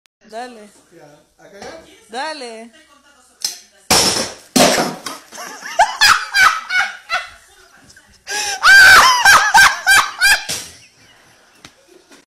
Pan handle breaks during flip
A guy cooking on a stove tries to flip a pan when the handle breaks and the base with food falls off.